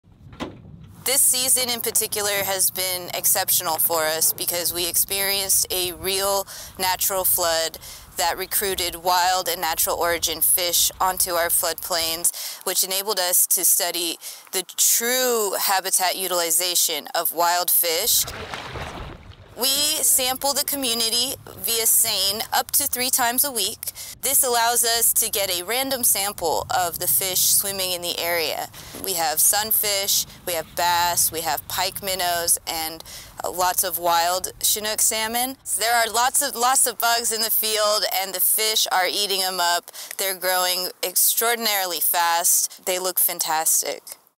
Bypass Flooding Brings Wild Salmon & Rice
A series of atmosphere river storms brought bypassing flooding — and dozens of wild salmon and other fish species — into Sutter Bypass rice fields now being studied by a team of UC Davis scientists.